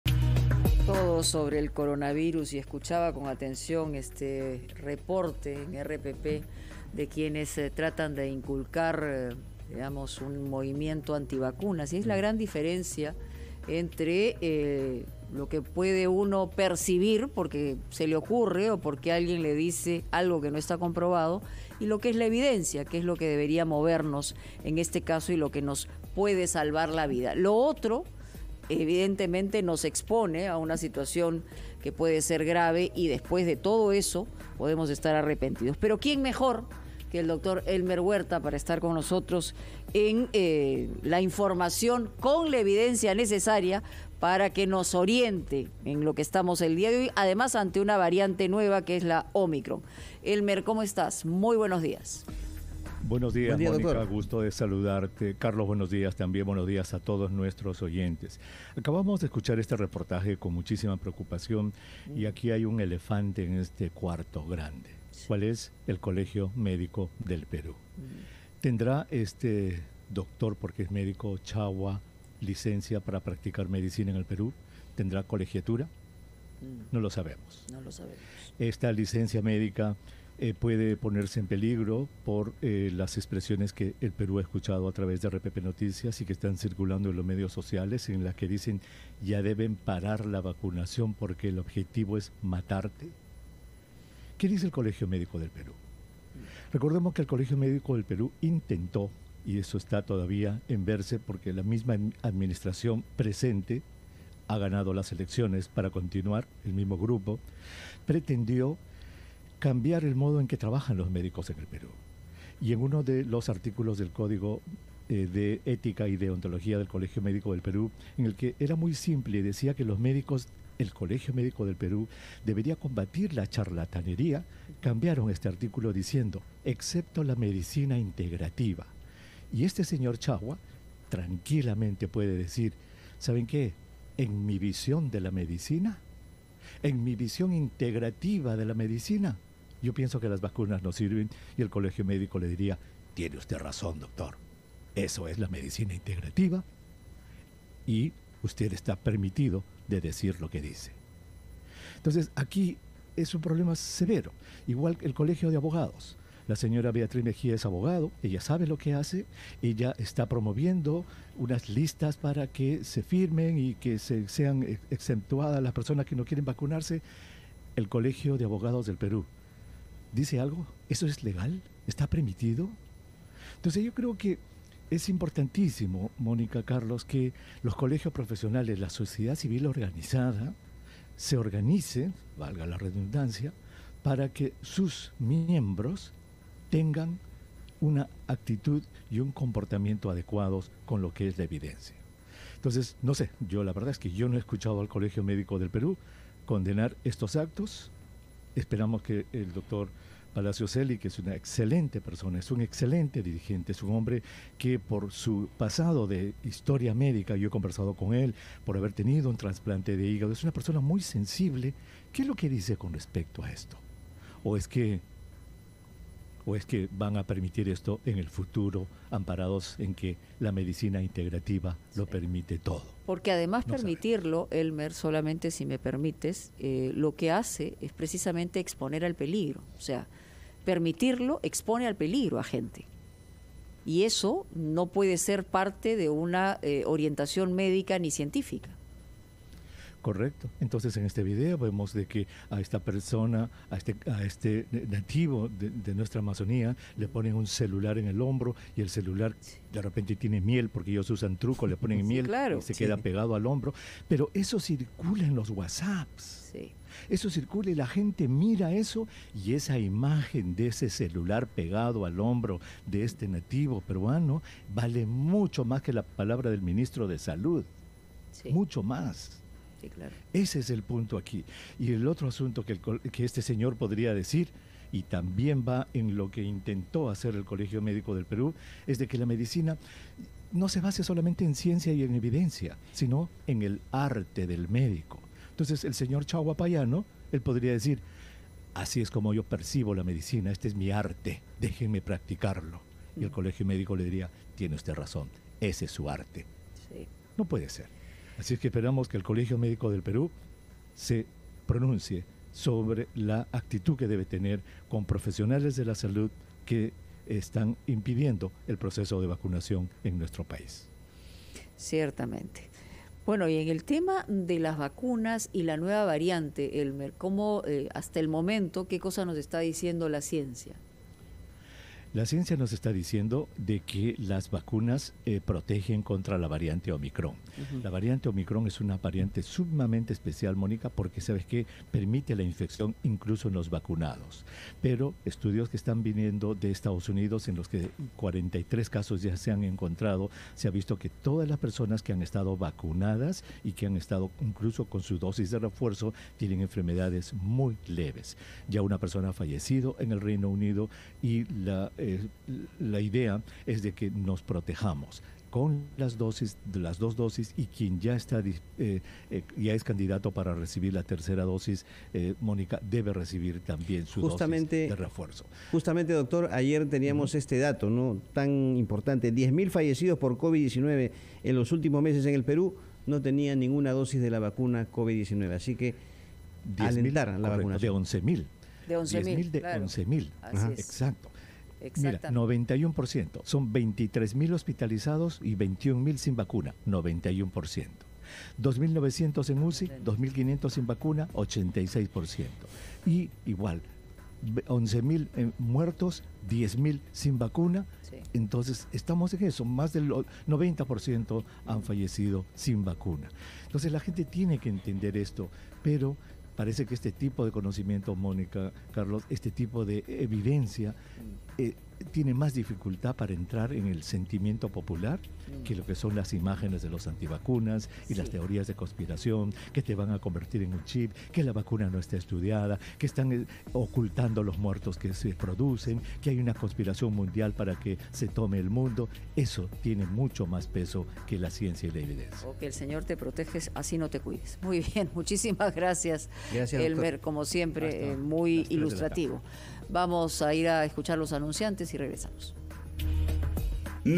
El Dr. Elmer Huerta explicó por qué hay profesionales de la salud promoviendo la no vacunación
El Dr. Elmer Huerta, consejero médico de RPP Noticias, explicó que la medicina integrativa "lo permite todo" y con esto varios profesionales de la salud se han respaldado para promover la no vacunación contra la COVID-19.